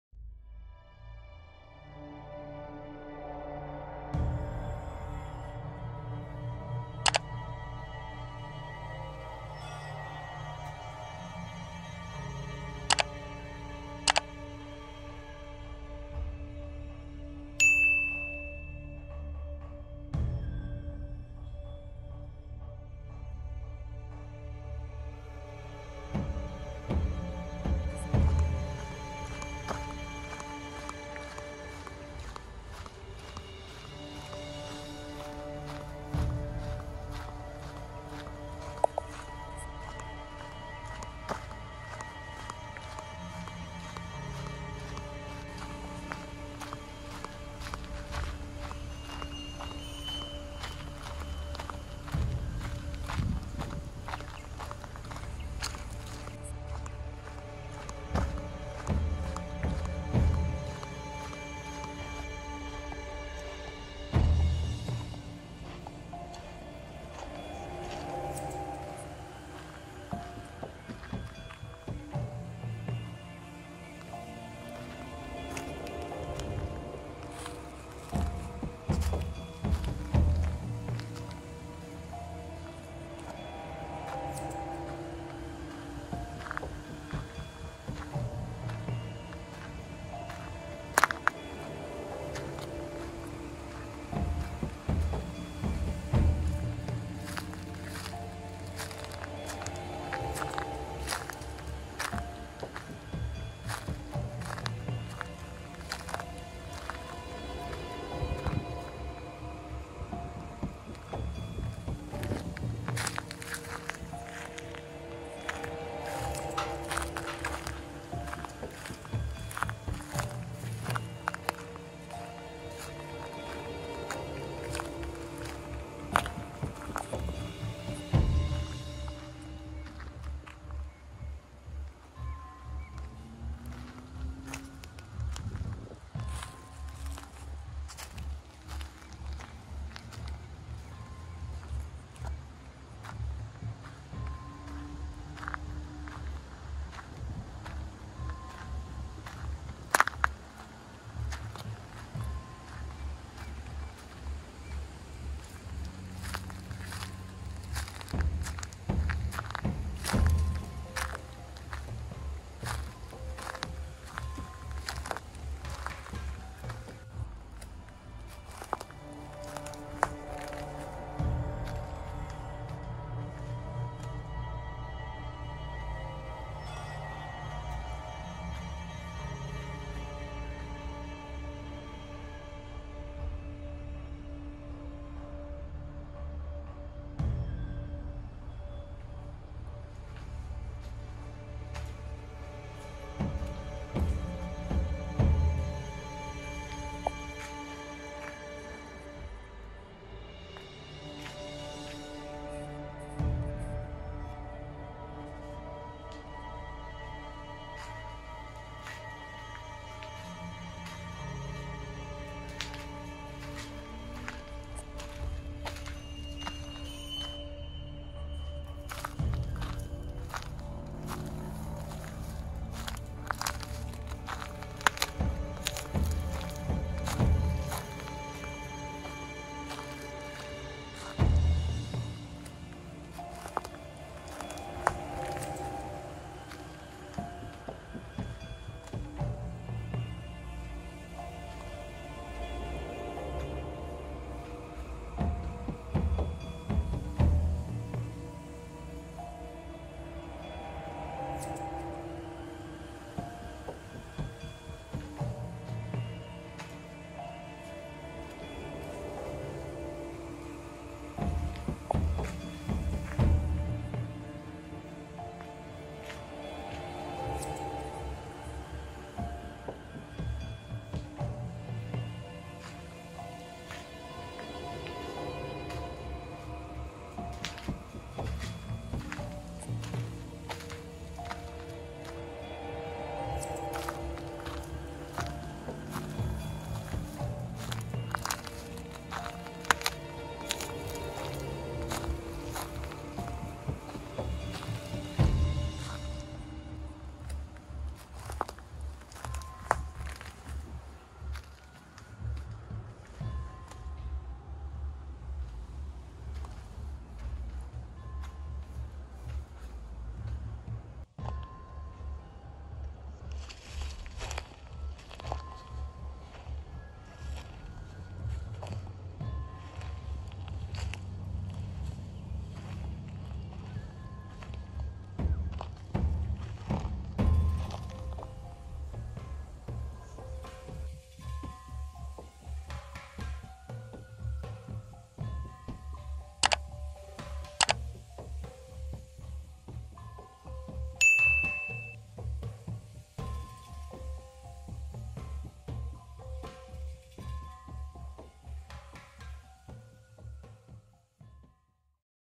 An Old Abandoned Derelict Shack i had a look at in Neerabup Western Australia 2020
Not much to see but curious why so in the middle of the bush.